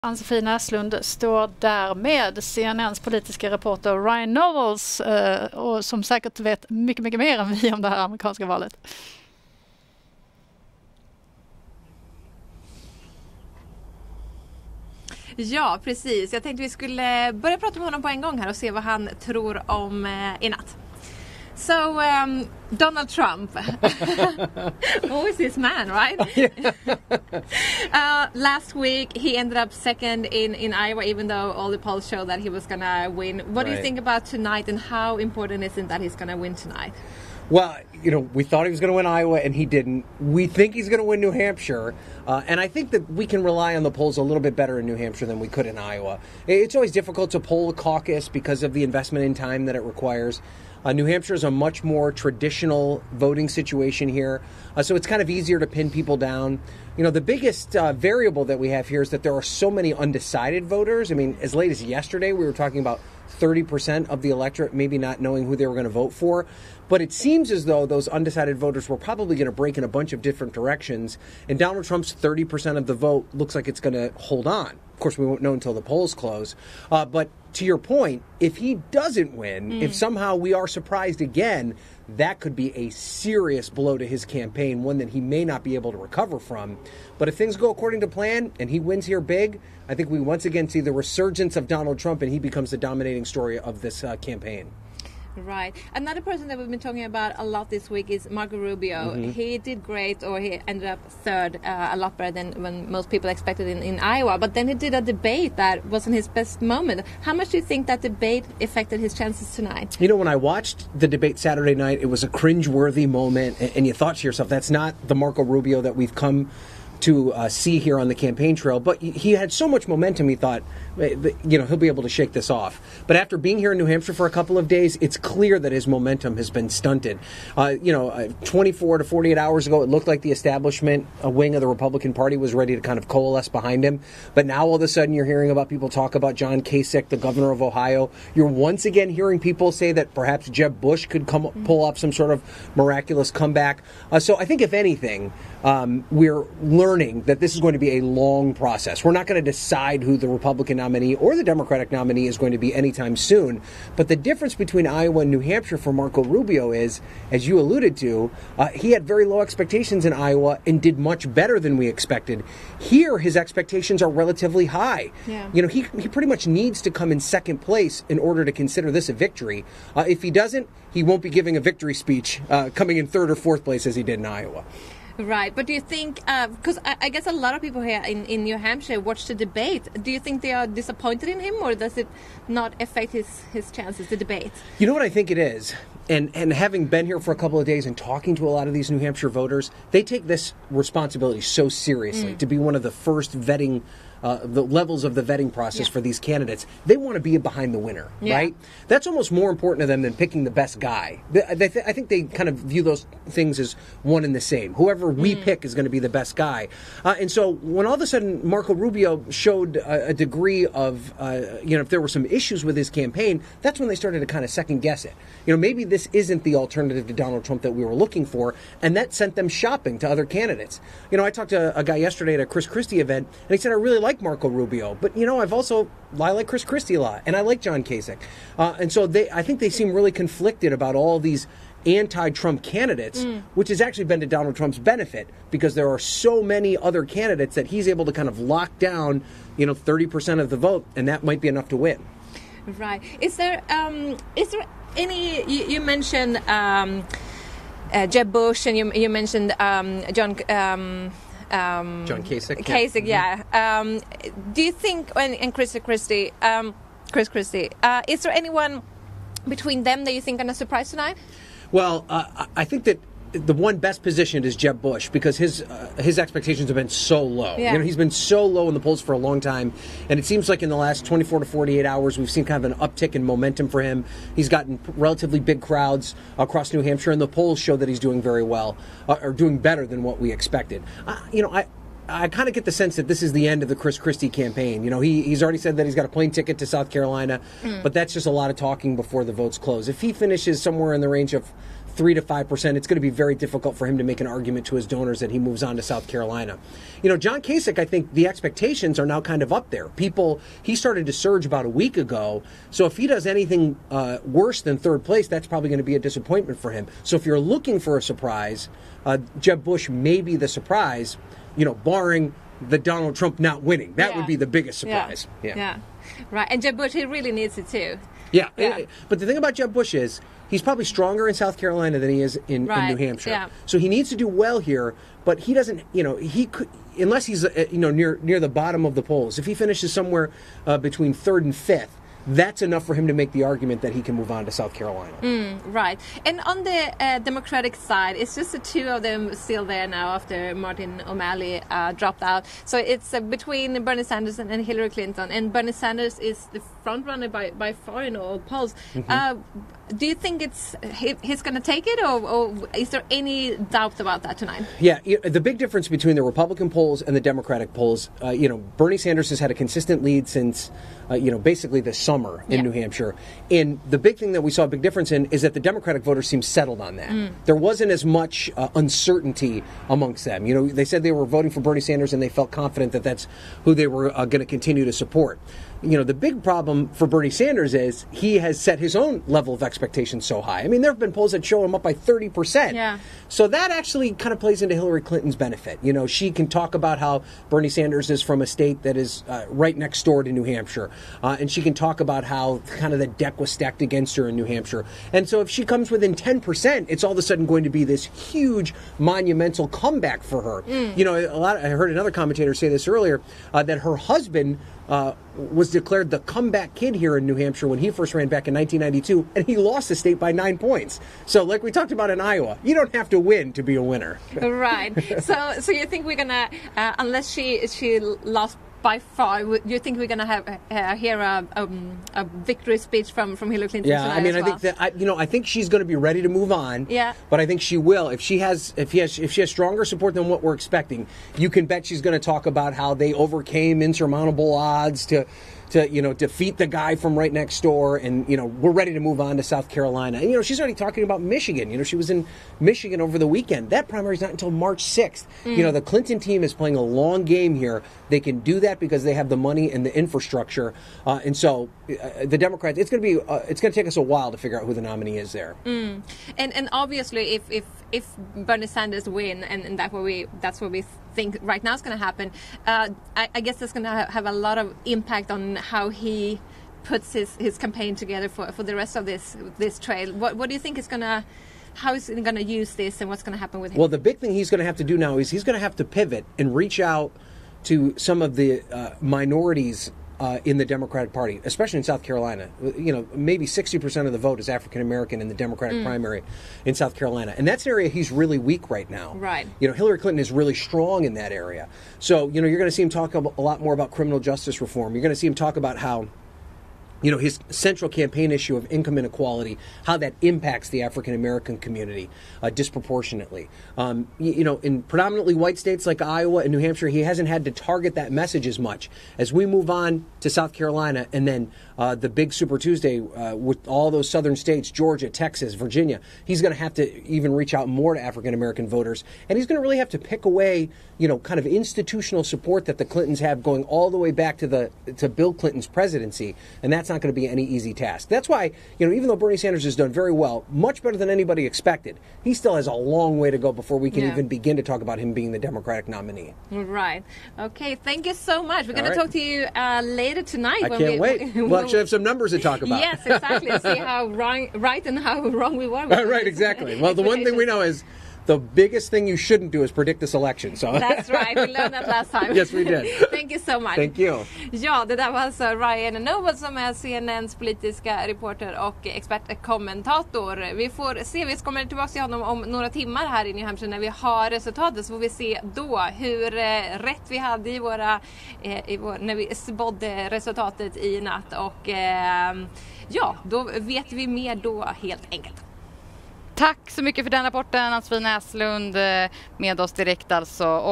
CNN-profilen Ryan Nobles om första primärvalet
Direkt från New Hampshire